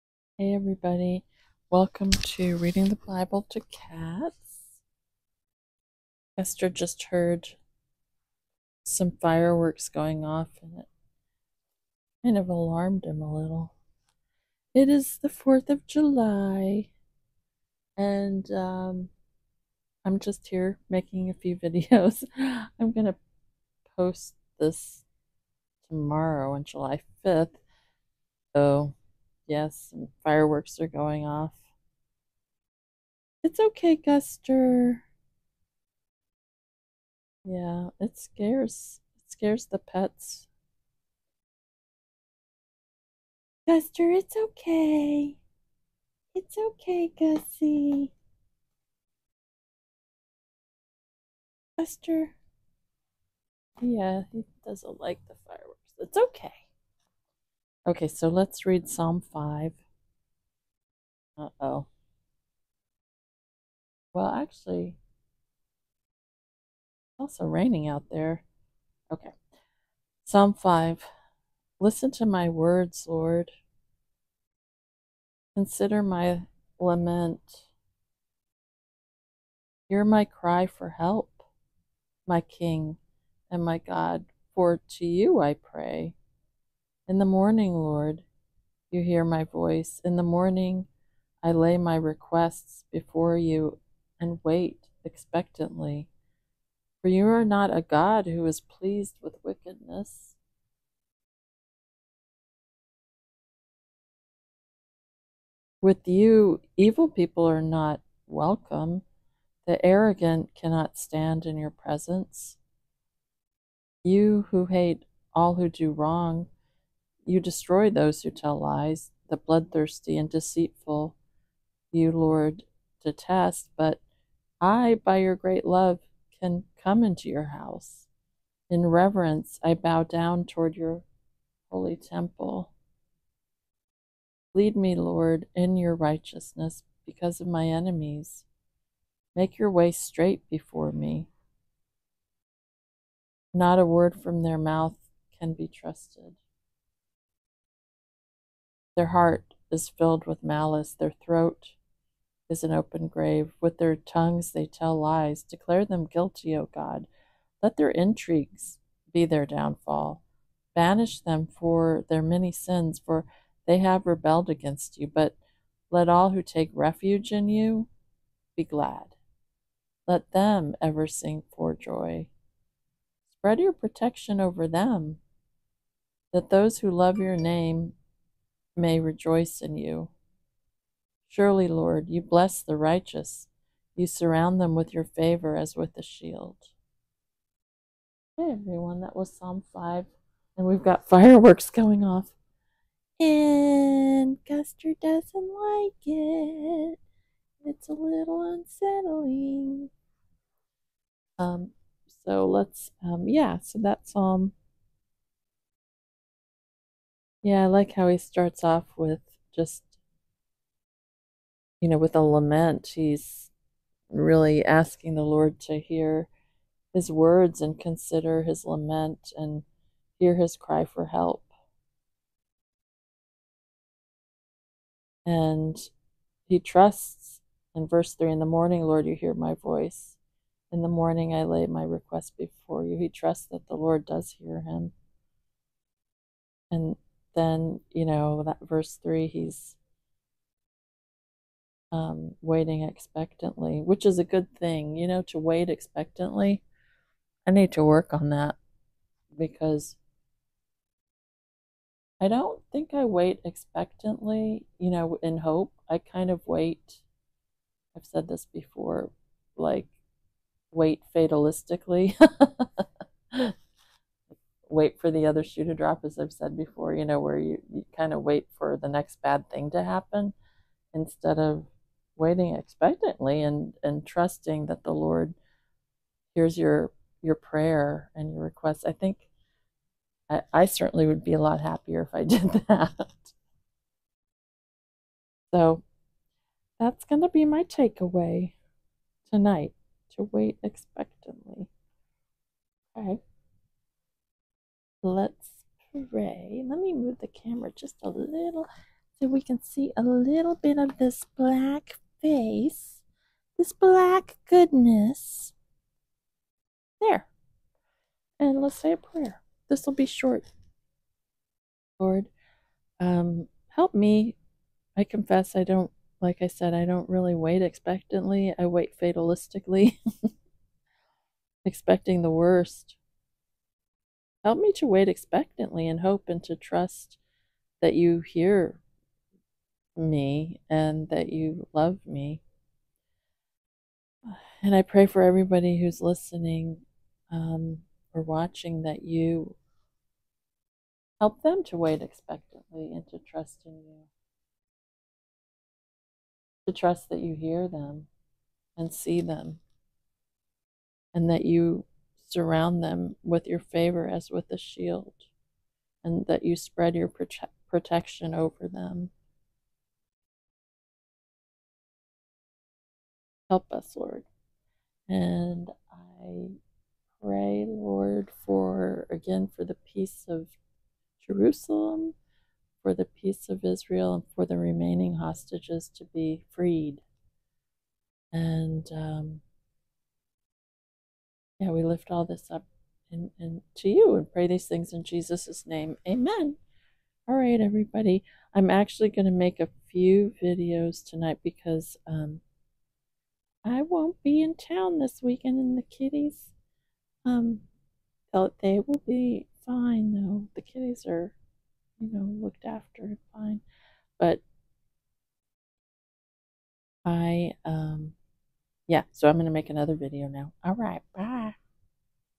Psalm 5
We read and talk about Psalm 5 while Gus looks cute 🥰